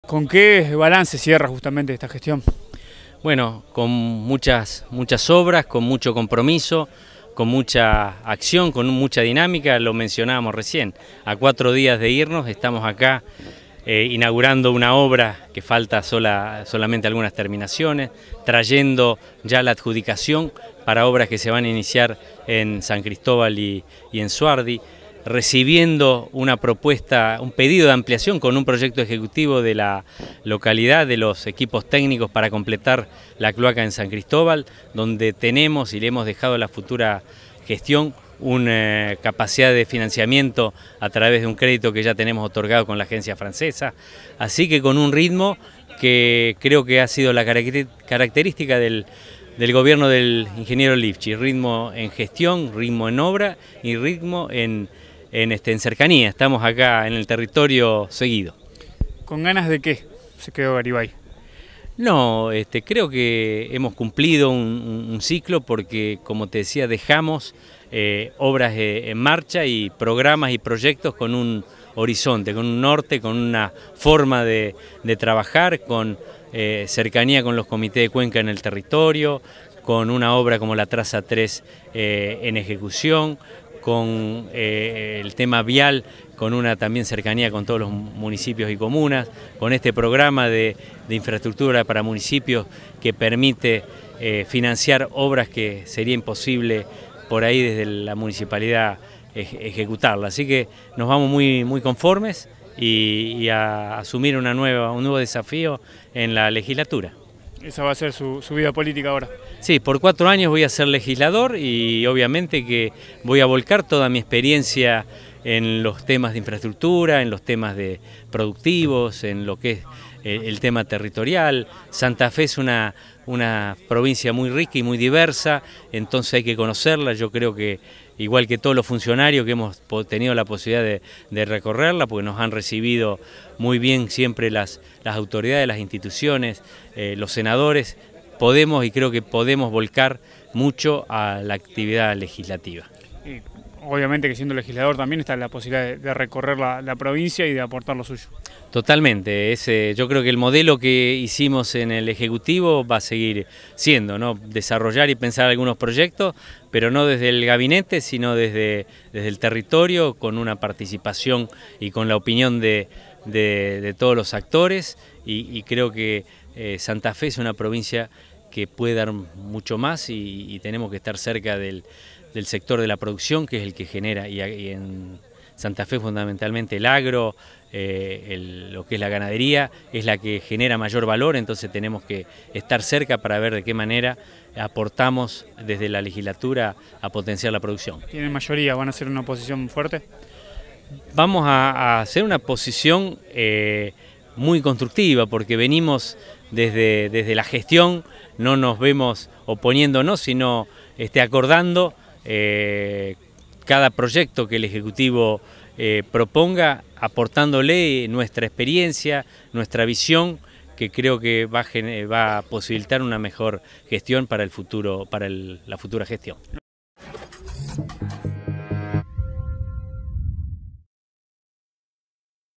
"Dejamos la vara alta"
Fue una de las frases del Ministro de Infraestructura y Transporte José Garibay, quien también estuvo presente en la inauguración de cloacas en el sector sureste de la ciudad. El Ministro destacó el trabajo que se a hecho a lo largo de la gestión del Gobernador Lifschitz en materia de obra pública, esperan que la vara alta que dejó este gobierno se mantenga.